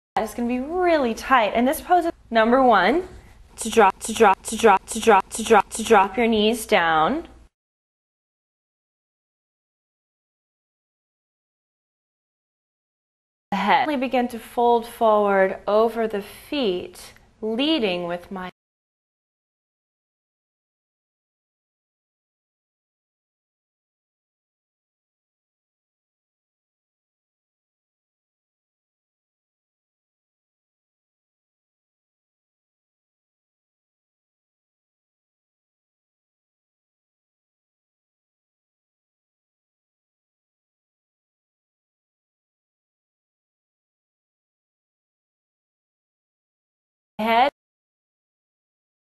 Best Parts of Yoga Teasing Girl Gets Lots Of Hits on Youtube
I found a girl demonstrating Yoga, and after reading the comments, it was clear that everyone agreed on the best parts of the video.